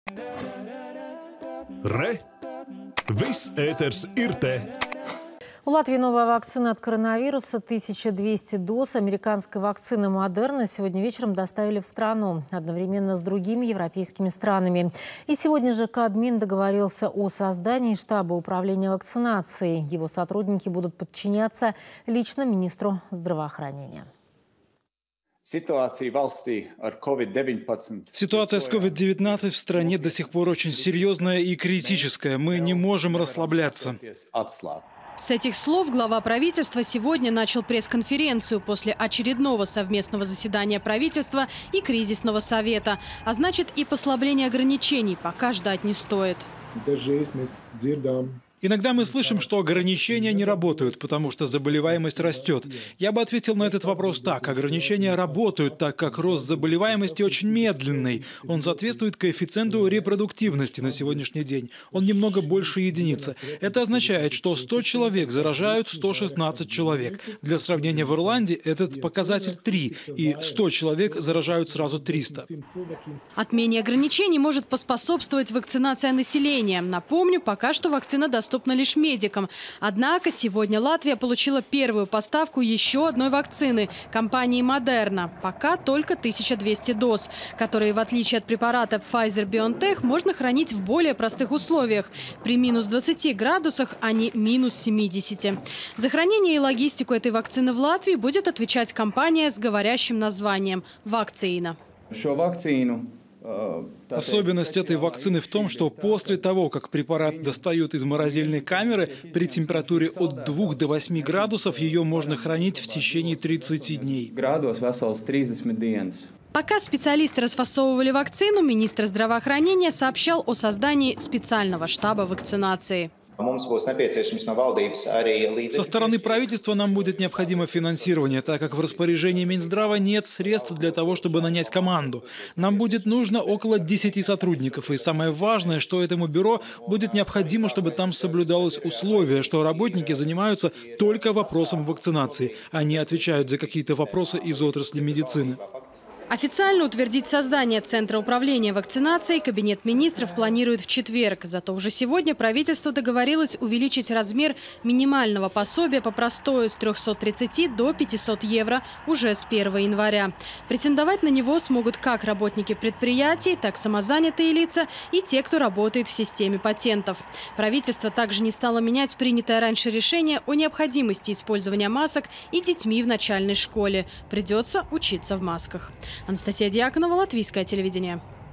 В Латвии вторая вакцина от Ковид-19
У Латвии новая вакцина от коронавируса. 1200 доз американской вакцины «Модерна» сегодня вечером доставили в страну. Одновременно с другими Европейскими странами. И сегодня же кабмин договорился о создании штаба управления вакцинацией. Его сотрудники будут подчиняться лично министру здравоохранения.